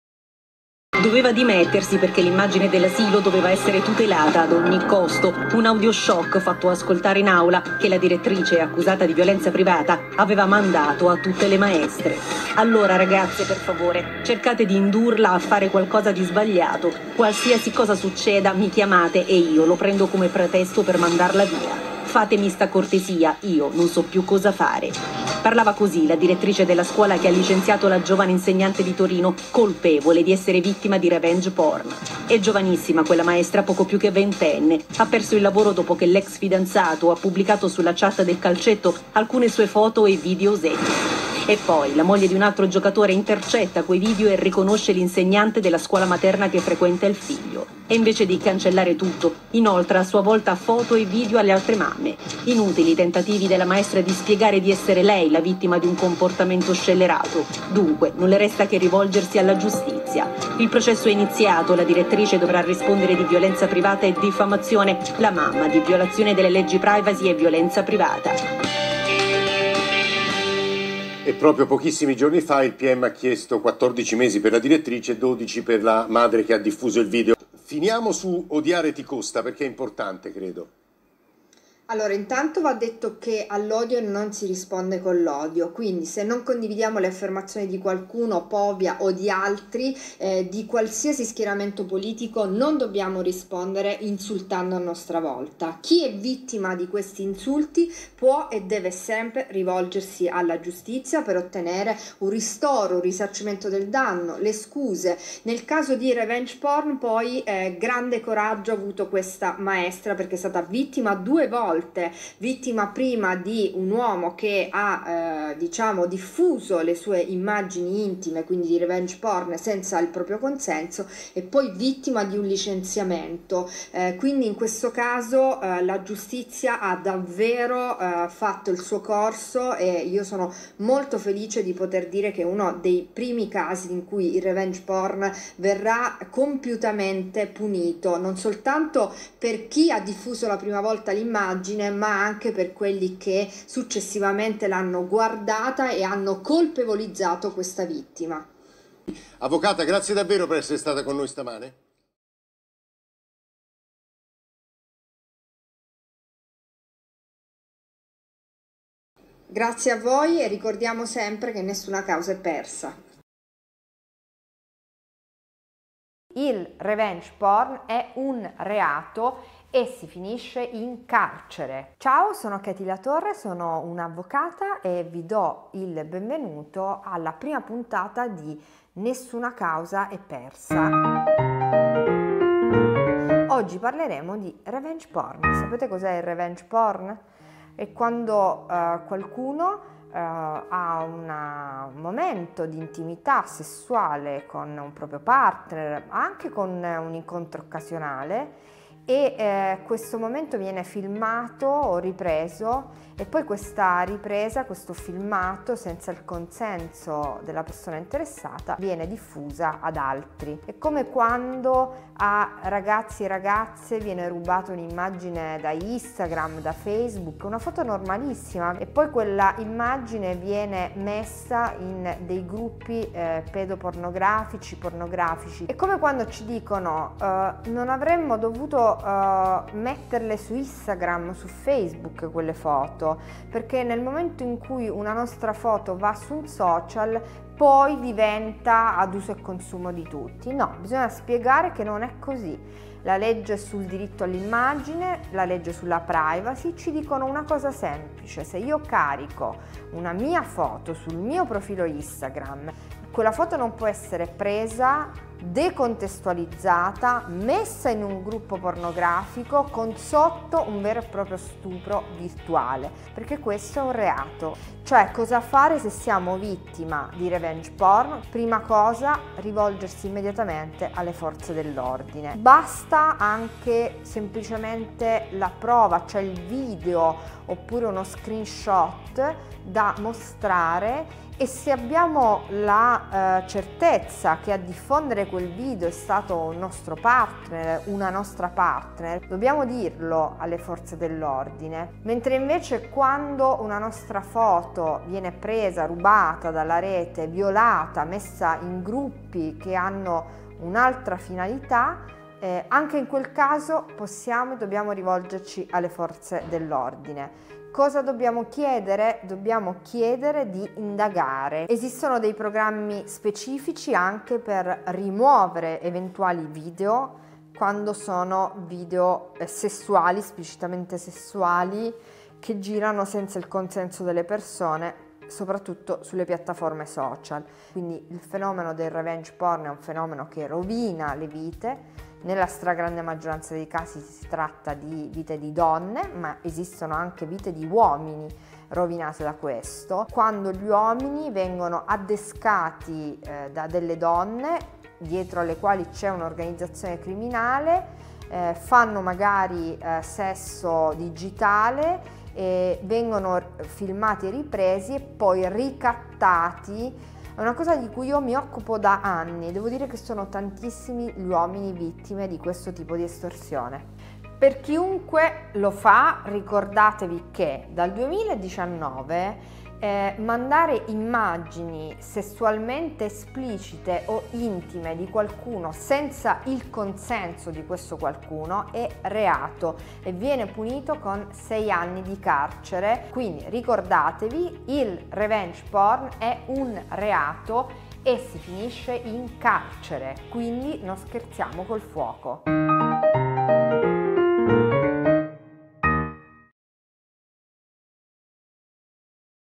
ll Revenge Porn
ll Revenge Porn è un reato! Si va in carcere! Non si scherza col fuoco.